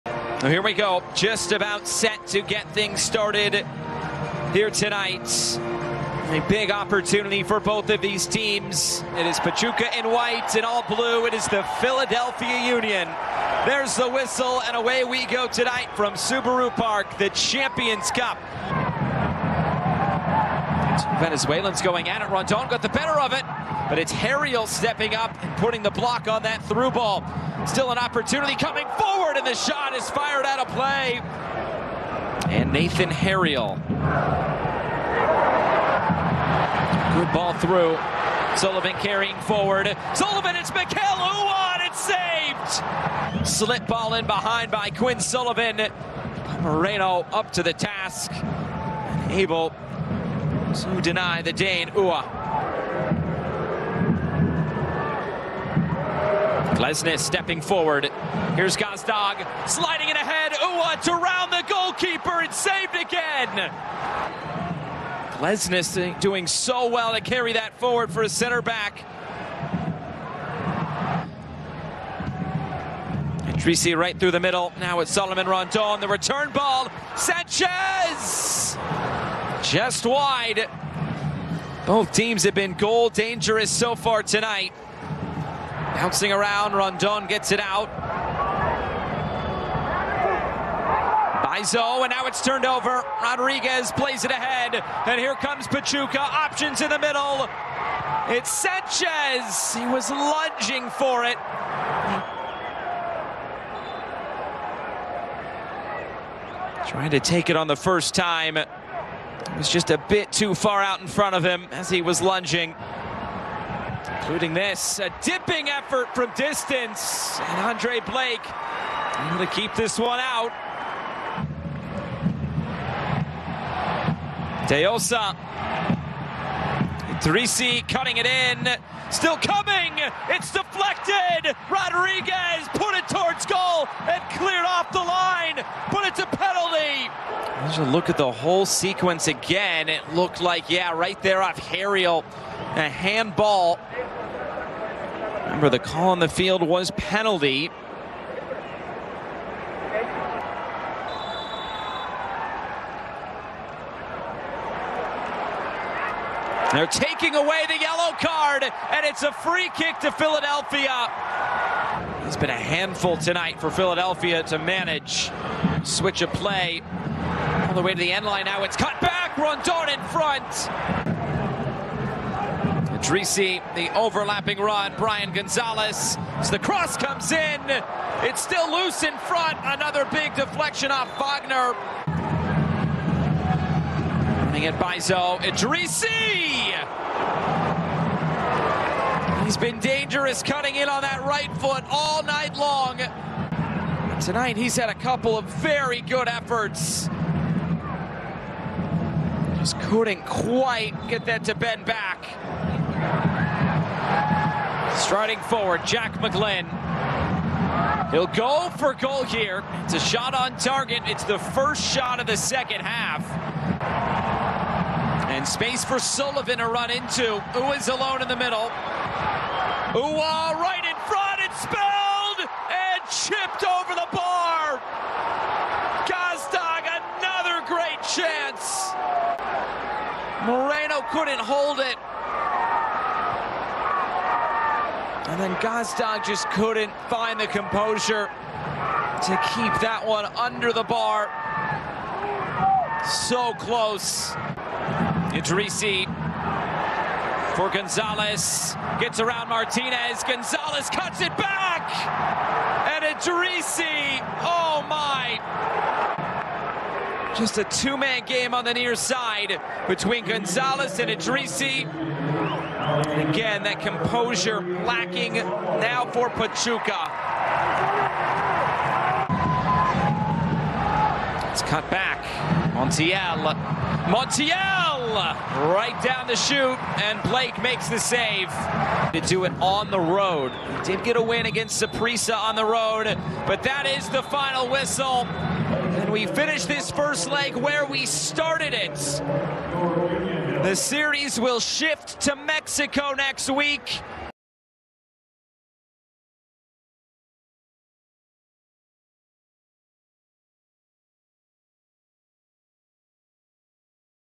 Philadelphia Union vs. CF Pachuca CONCACAF Champions Cup Highlights | FOX Soccer
Philadelphia Union and CF Pachuca squared off in the Round of 16 CONCACAF Champions Cup! Check out the top moments and highlights here!